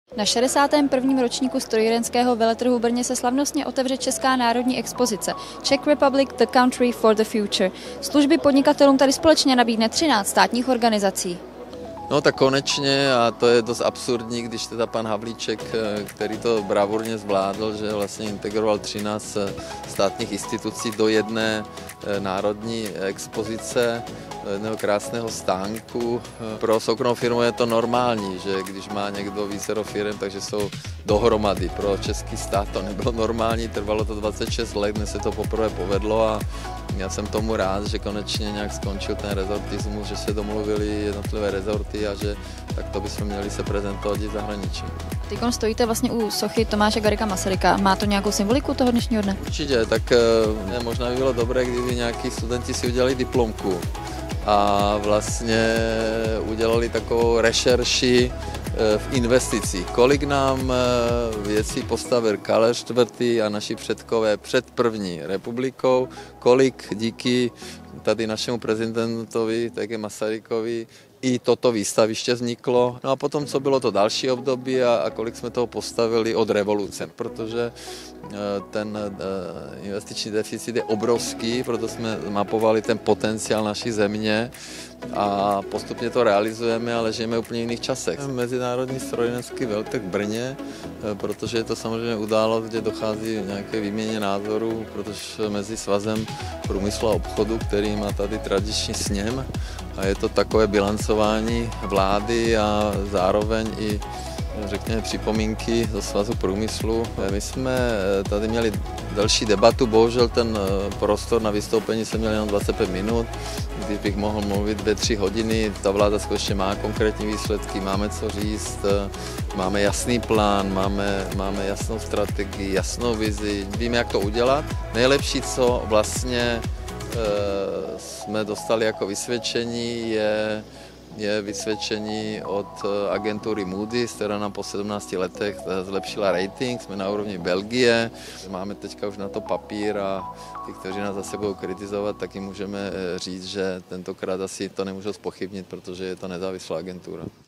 Strojírenský veletrh 🇨🇿
Ještě k Brnu. Jak jsem tam byl od rána do noci na strojírenském veletrhu. Koukněte 🤓  Konečně se nám po 26 letech podařilo, aby se třináct důležitých státních institucí spojilo. Super! A abychom spolupracovali napříč ministerstvy a vystoupili tam jednotně. A je jedno, z jaké jsme politické strany. Velký dík za to patří hlavně ministrovi průmyslu Karlu Havlíčkovi, který tenhle úkol zvládl splnit za půl roku. Jsem moc pyšný na Českou národní expozici Czech Republic: The Country For The Future, u jejíhož slavnostního otevření jsem mohl být.